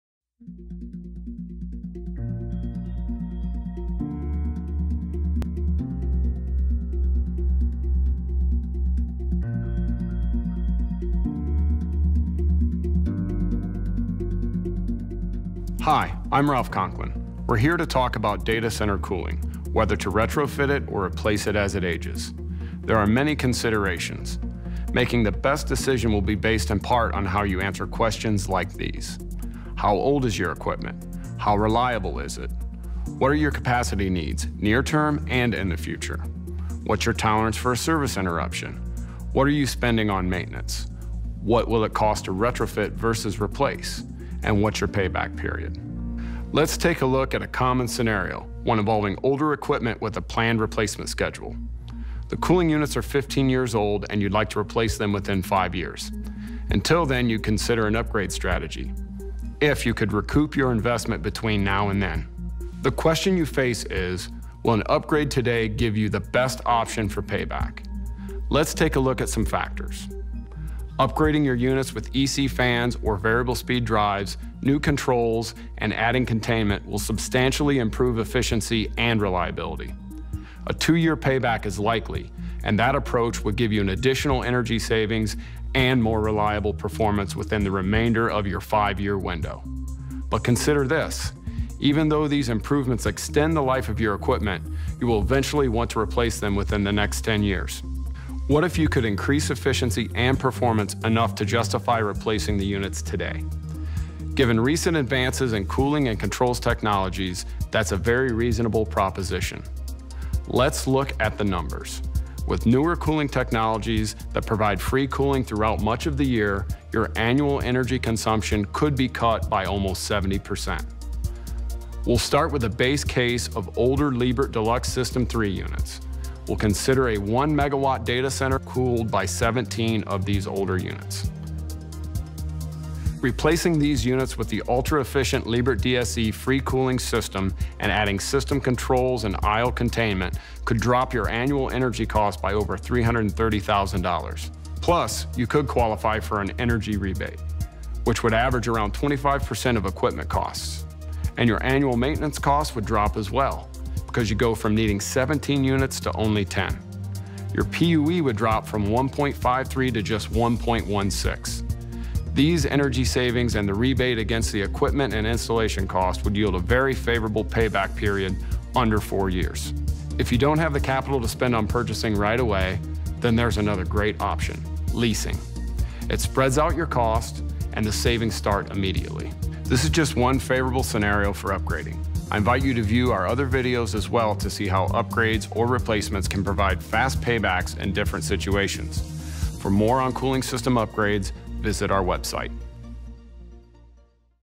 Benefits of Updating or Replacing Old Data Center Cooling Equipment On A Replacement Schedule
In this video, Vertiv shows you two options for improving data center cooling performance: upgrading aging data center cooling equipment already in a replacement schedule with more efficient cooling equipment to save more than $135,000 in annual energy costs, reduce your PUE and see a payback on your investment in under 2 years; or replacing the equipment to save $330,000 in annual energy costs with energy efficient data center cooling equipment, rack cooling equipment and aisle cooling equipment with Vertiv data center cooling solutions and see a payback in under four years.